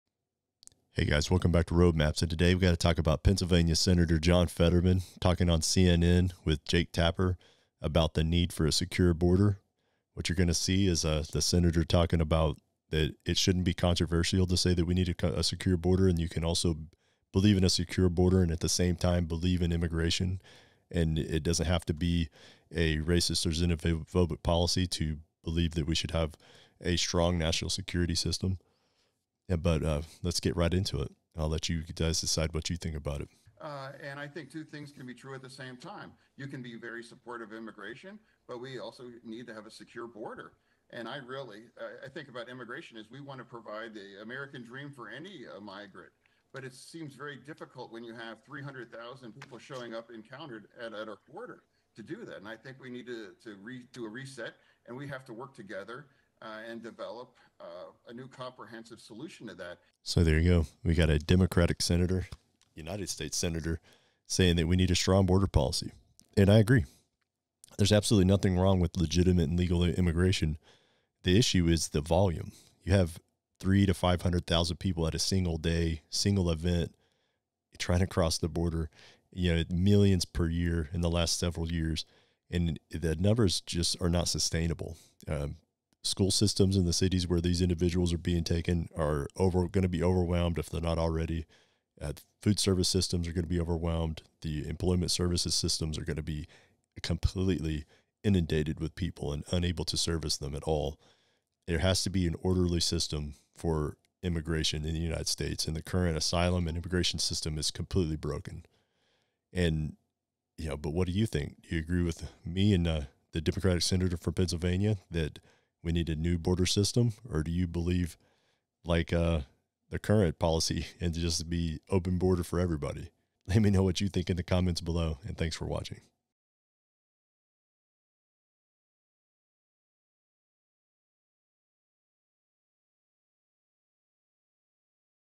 Democrat says Illegal Immigration Threatens Working Class Americans.
U.S. Senator John Fetterman agrees that it is not sustainable to have millions of illegal border crossings per year.  In this short episode you'll hear Senator Fetterman give his opinion (and mine) that immigration should be supported but it has to be orderly and controlled.  A strong national security system is a must in today's world and that starts with a strong border policy.  The asylumseekers policy in the United States is broken.  The bordercrisis is going to, and already has in some places, strain our schools and other services.  The outcome of the election2024 and whether
or biden is President will determine this issue. Photo Credit:  Edgar H. Clemente (AP news) and JohnFetterman.com

Original Source:  ALX 🇺🇸 on X: "FETTERMAN: “I don’t understand why it’s controversial to say we need a secure border. We want to provide the American dream for any migrant, but it seems very difficult when you have 300,000 people showing up at our border to do that.” / X (twitter.com)